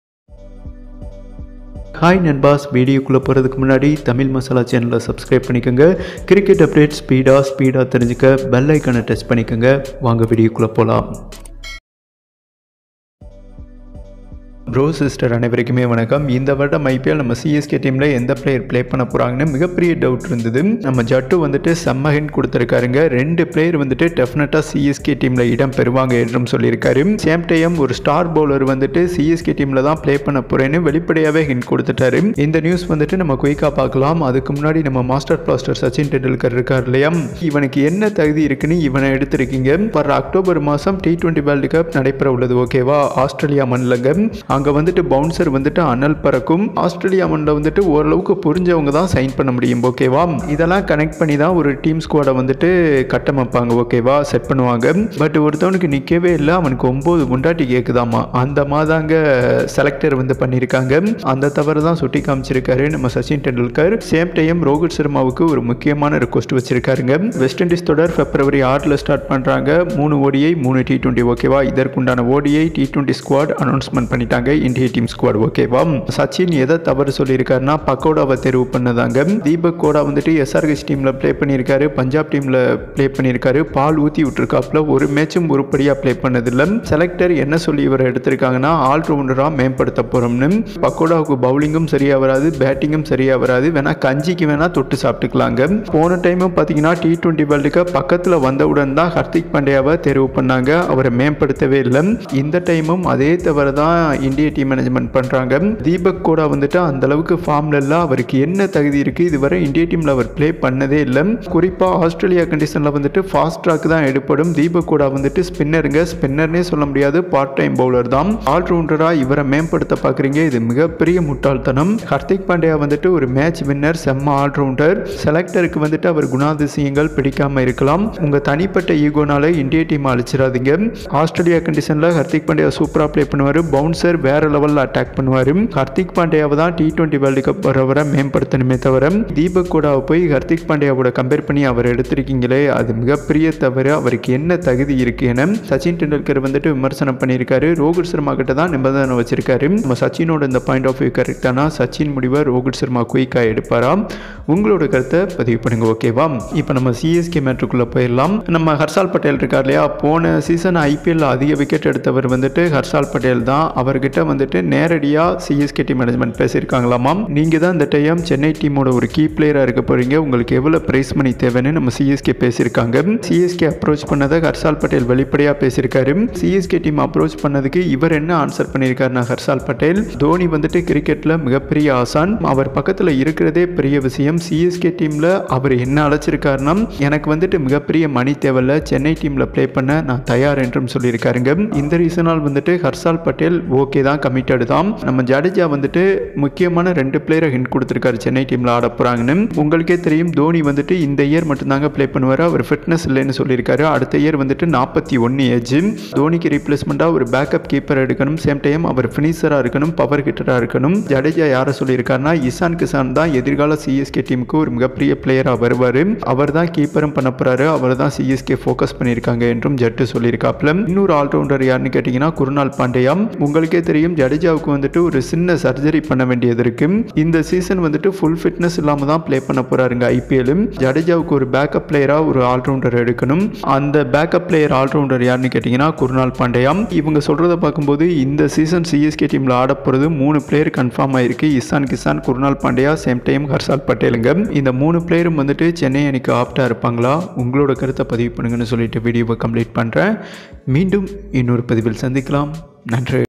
ஐபிஎல் 2022 : தோனிக்கு மாற்றுவீரர் இவர்தான்! ஜடேஜா பரபரப்பு தகவல்! சச்சின் ஆவேசம்
India vs West indies Series 2022 | India Team Squad ! Sachin Tendulkar Angry Report